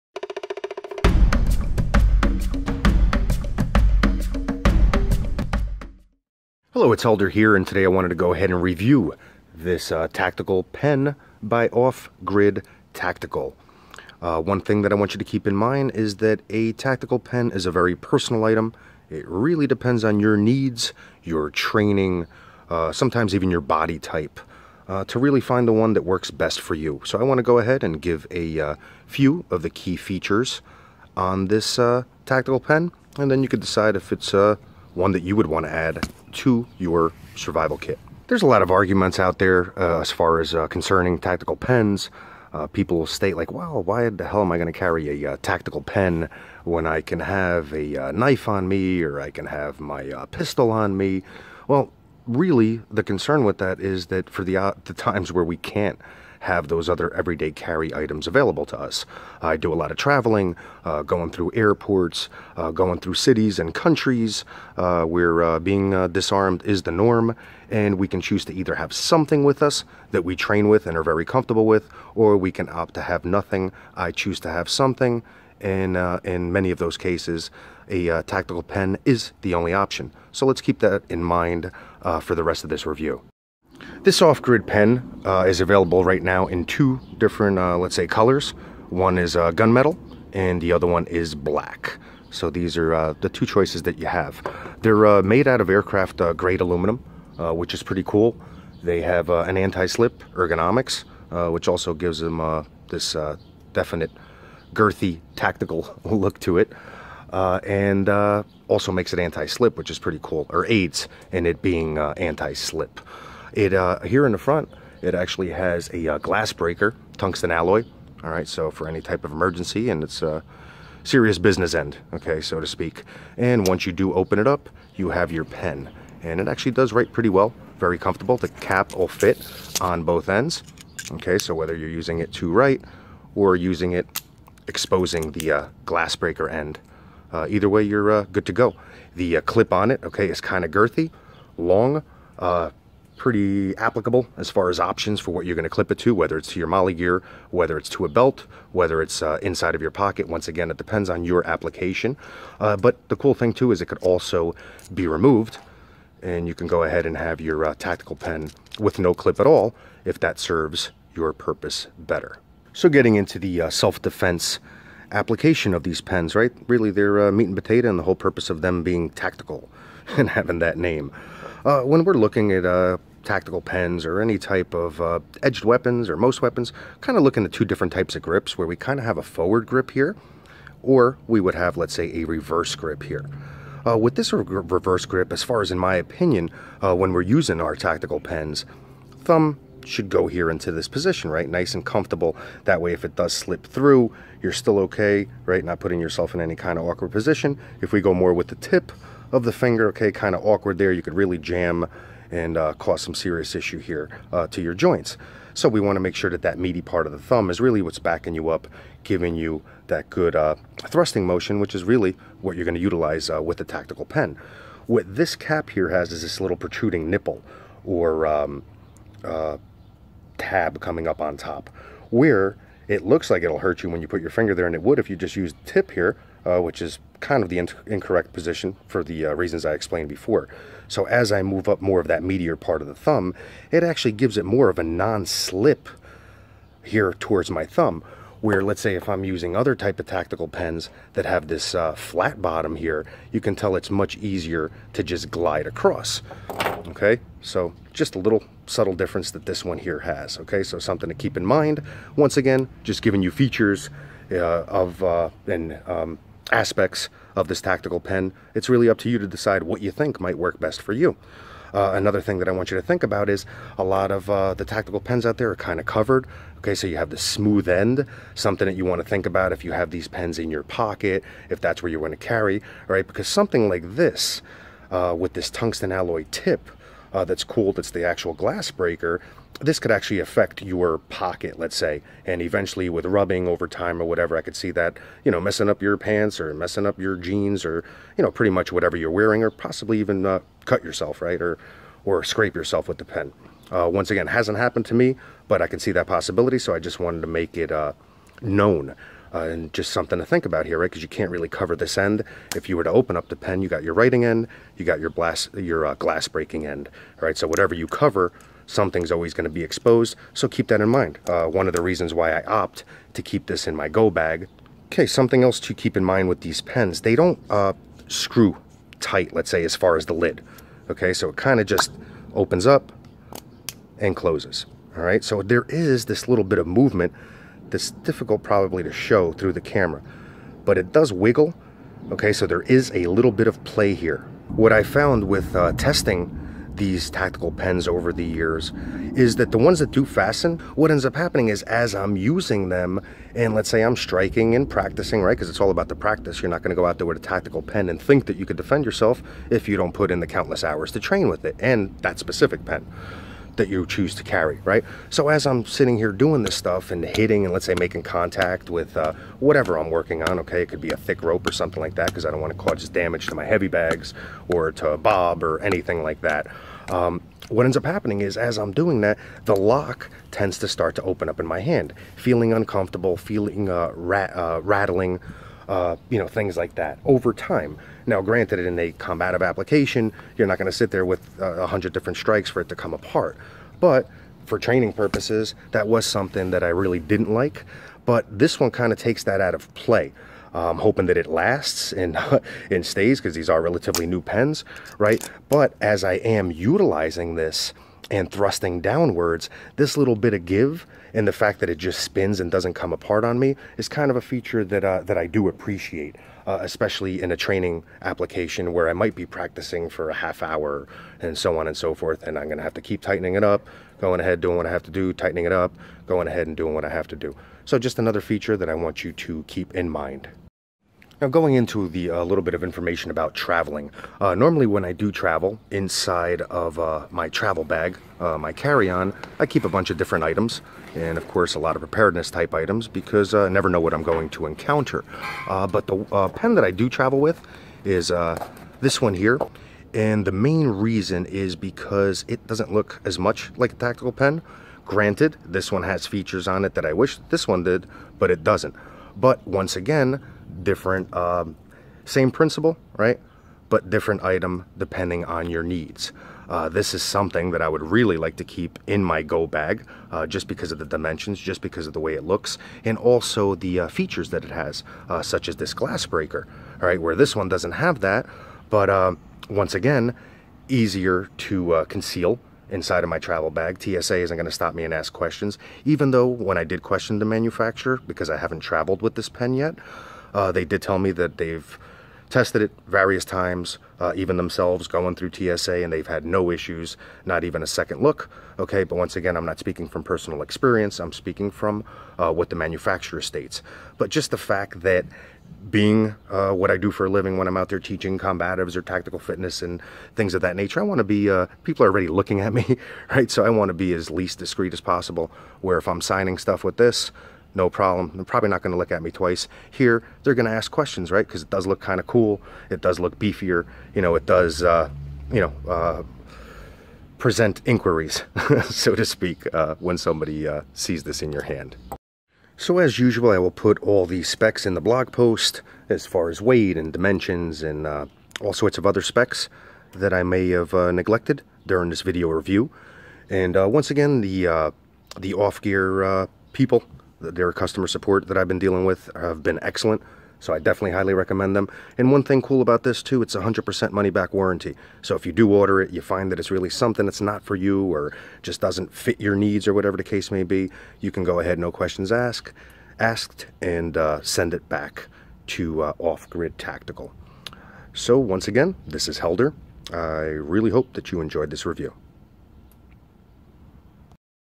Off-Grid Tactical Pen Review For International EDC
Coach Helder Reviews the Off-Grid tactical pen and explains it's features & benefits.